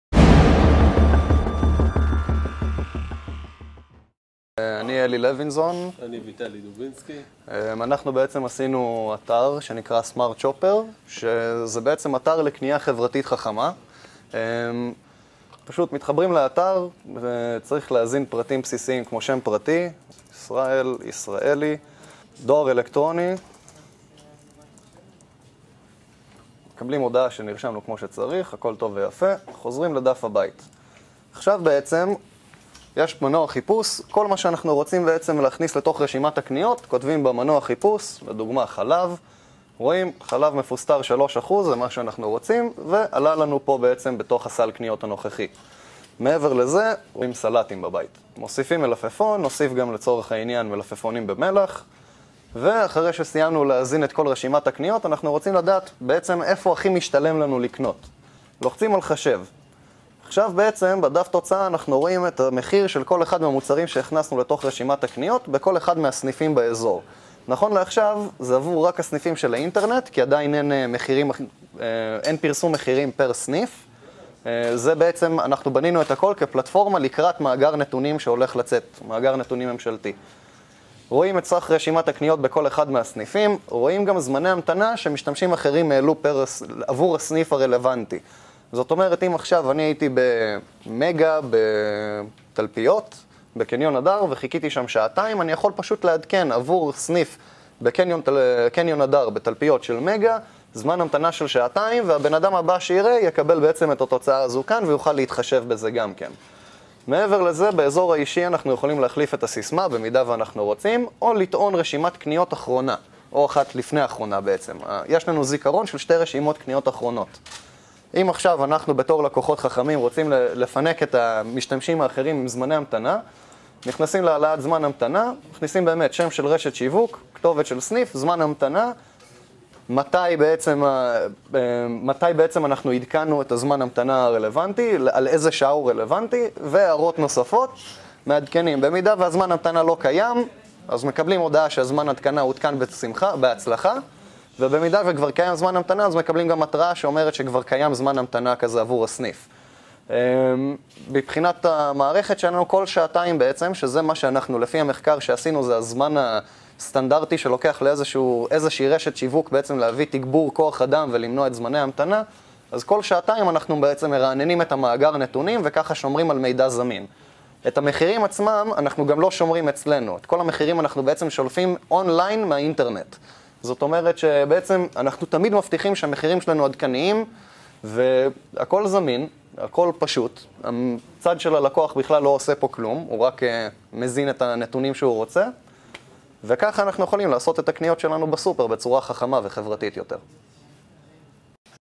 Smart Shopper
ויטלי דובינסקי ואלי לוינזון

אתר אינטרנט שהופך את הקניה ברשתות המזון ליעילה, זולה ונוחה. המשתמש מזין רשימת קניות ומקבל השוואה של מחיר הסל בסניפי רשתות מזון שונות ברדיוס גיאוגרפי מסוים. המידע מאפשר למשתמש לבחור את הסניף המועדף מבחינת זמינות המוצרים ועלותם. בהמשך, עם השקת מאגר המידע הממשלתי למחירי מוצרים ברשתות מזון, מחיר הסל ישוקלל עם המרחק של הצרכן מהסניף, עלות הדלק וזמני ההמתנה בסניף. הפרויקט מתבצע בשיתוף עם חברת Fusion Studio.